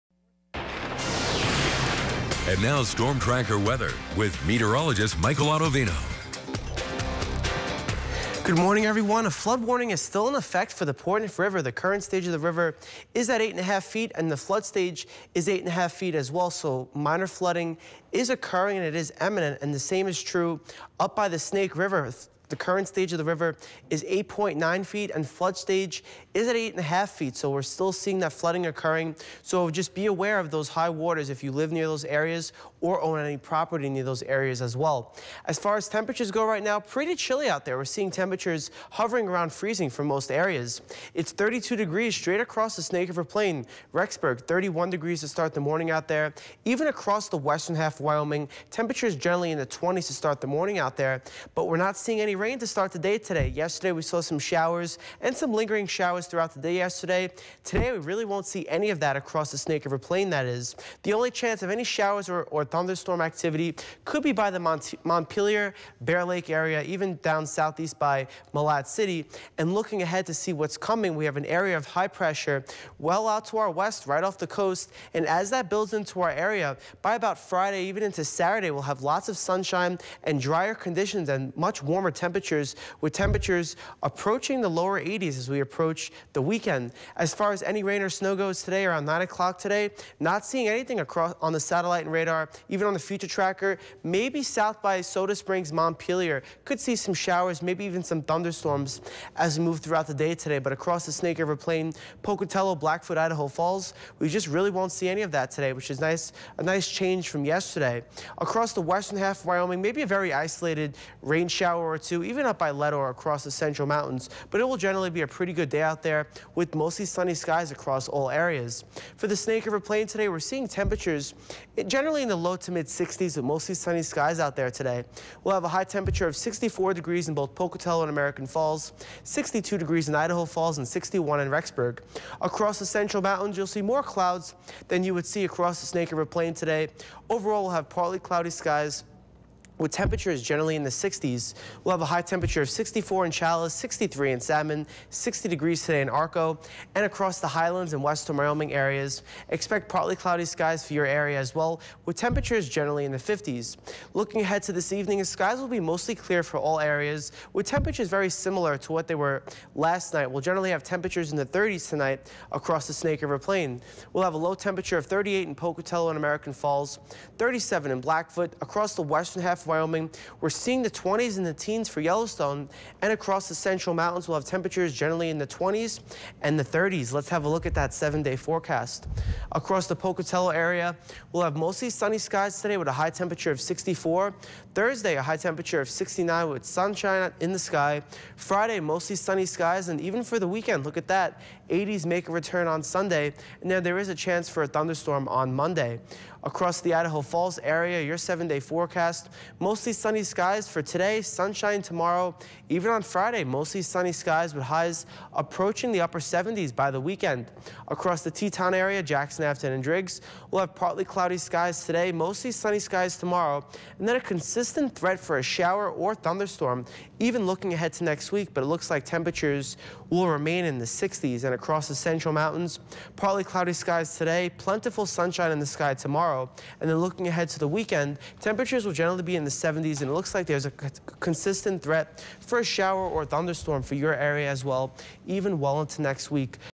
Meteorologist Michael Autovino's Morning Forecast For 5-2-2018
Skies will be mostly sunny across the Snake River Plain today with highs in the 60s.